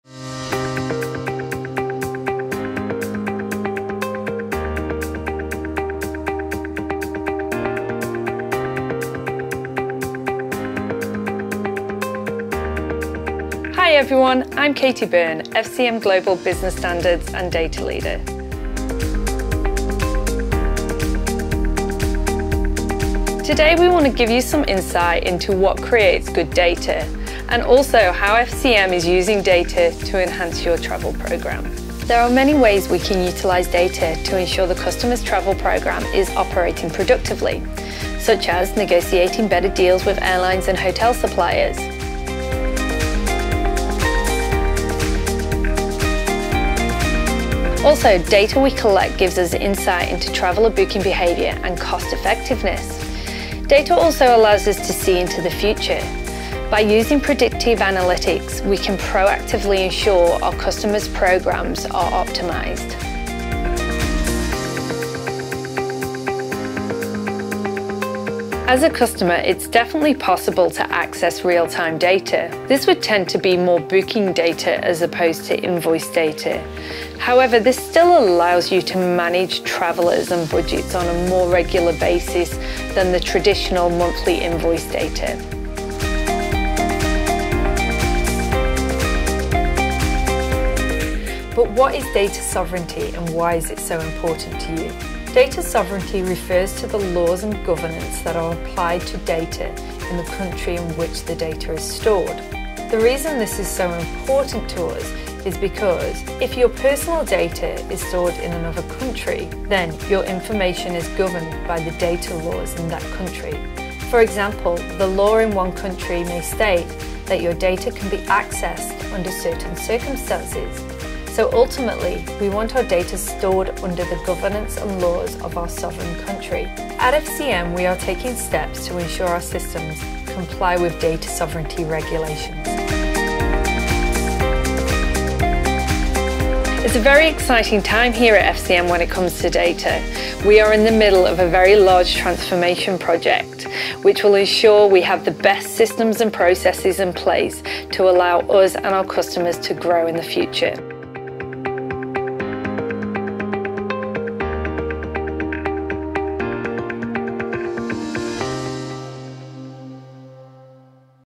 FCM: using data to ensure a productive corporate travel programme
From negotiating better deals with airline and hotel suppliers, to providing insight into traveller booking behaviour and cost effectiveness, good data is key to any corporate travel programme. Data provides a window to the future and through using predictive analytics FCM proactively ensures our customers' programmes are optimised.